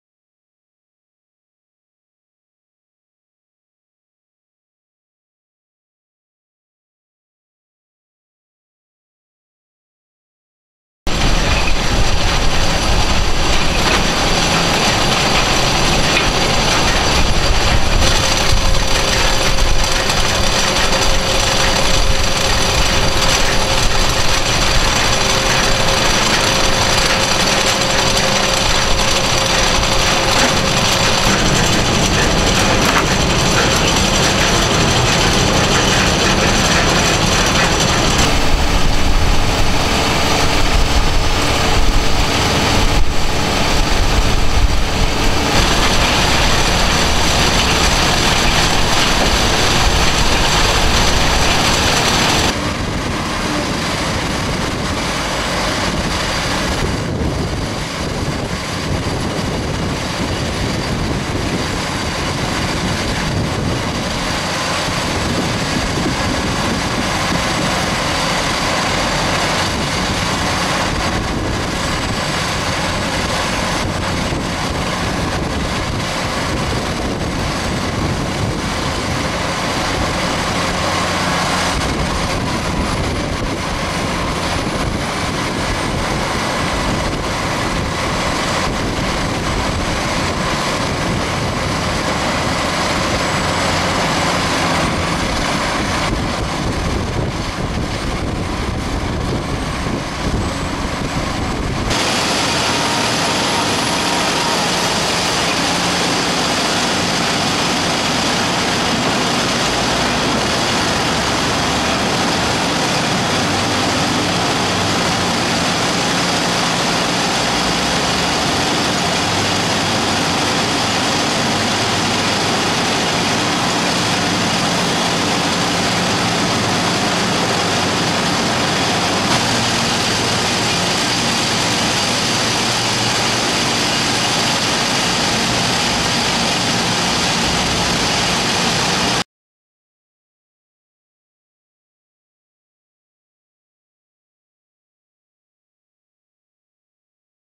Beet Shuttle by Kringstad Ironworks
36 Ton Beet Cart form Kringstad Ironworks Inc. Sorry the video got a bit choppy, we are working on editing the footage.